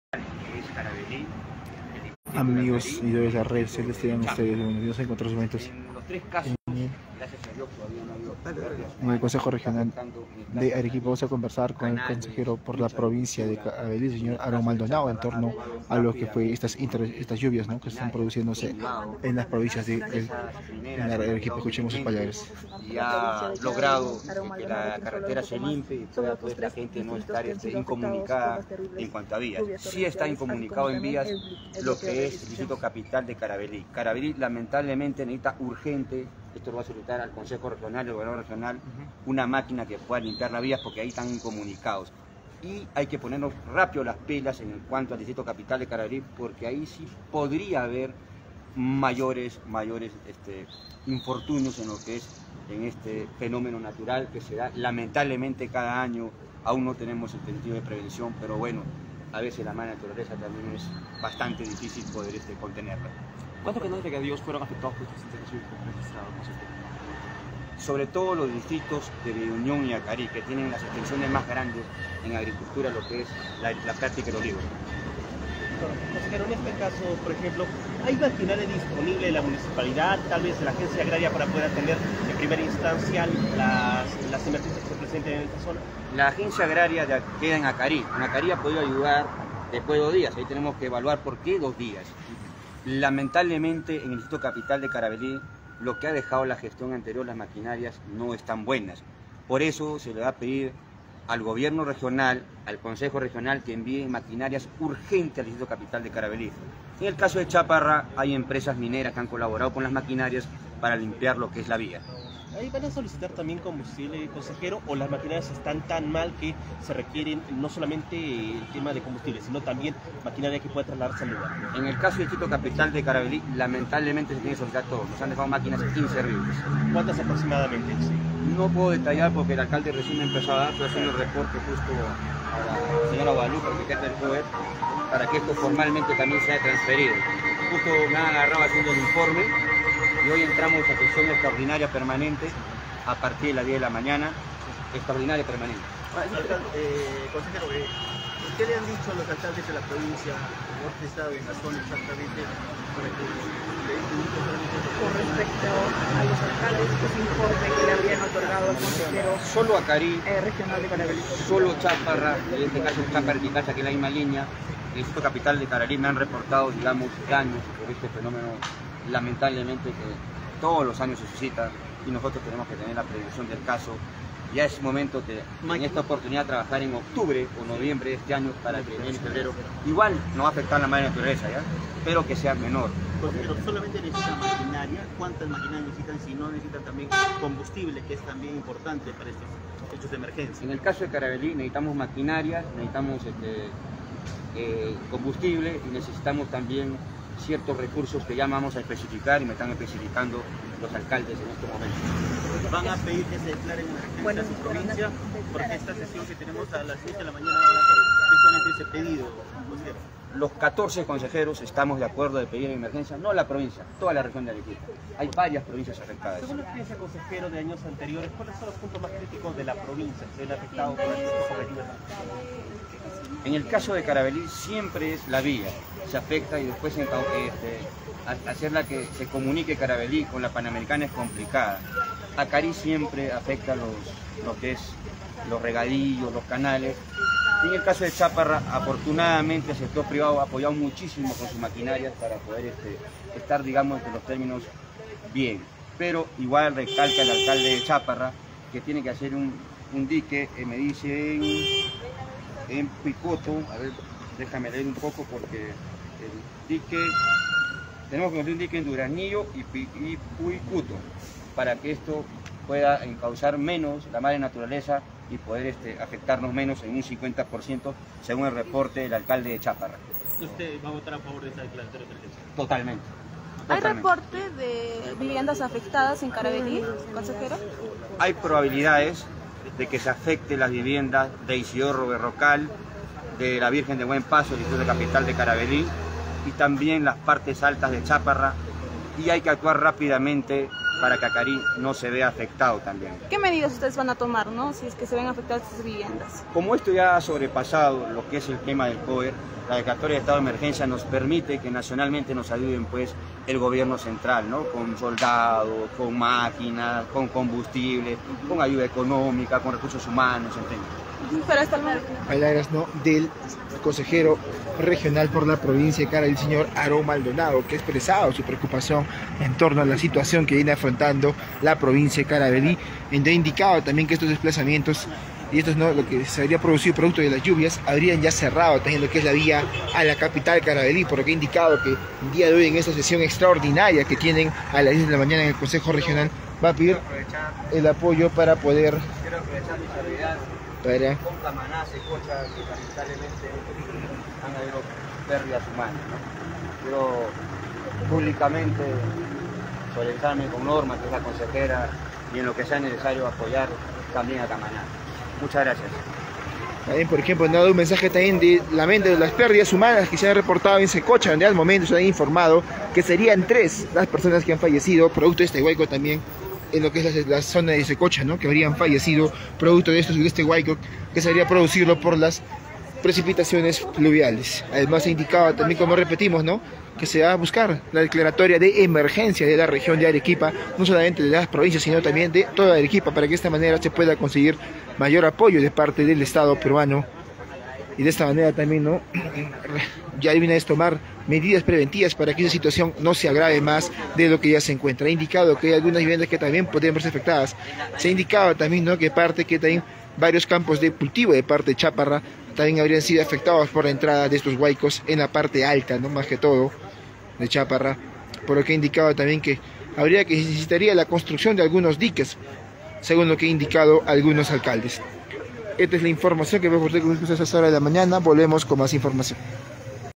Acari y Chaparra son las zonas más afectadas por las lluvias
Declara el Consejero Regional por Caravelí, Aaron Maldonado López